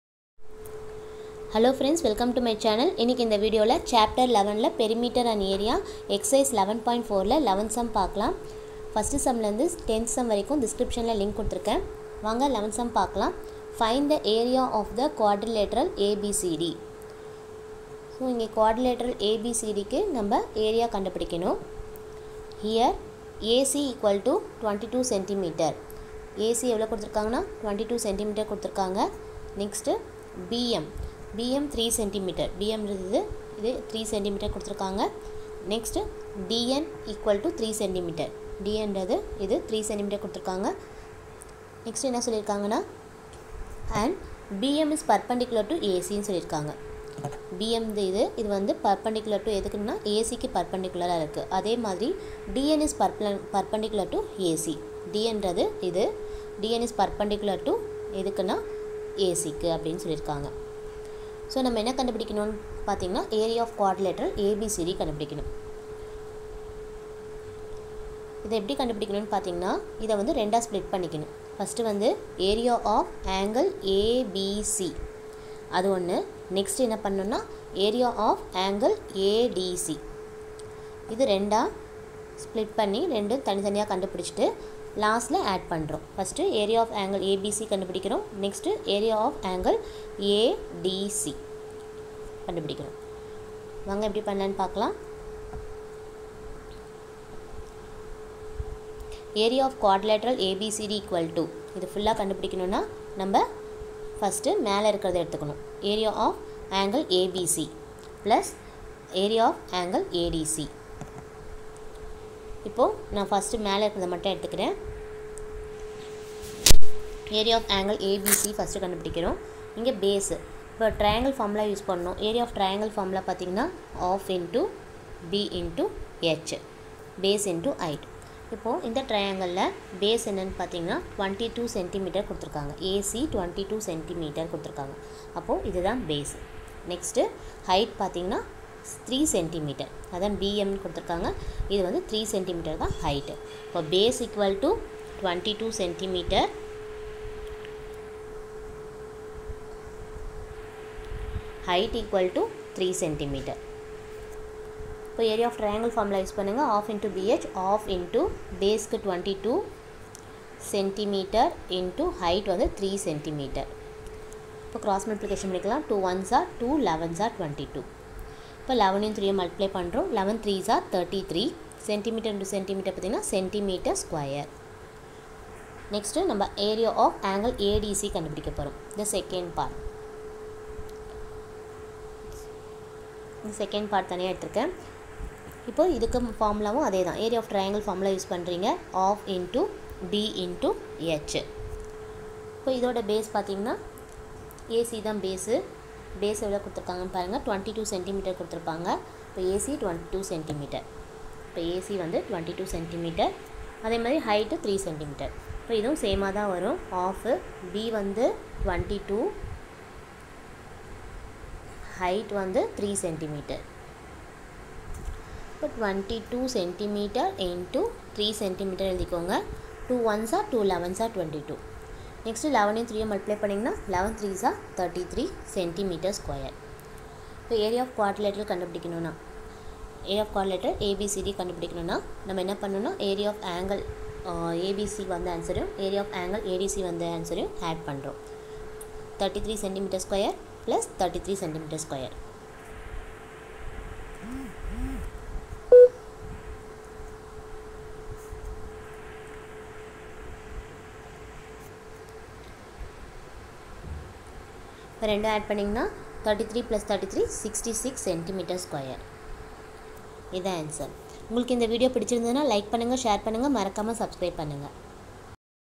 Perimeter and Area | Chapter 11 Ncert class 7 maths Exercise 11.4 Q(11) | Tamil explanation
Chapter 11 Ncert class 7 maths Exercise 11.4 Q(11)
Tamil explanation
Time for Kanidham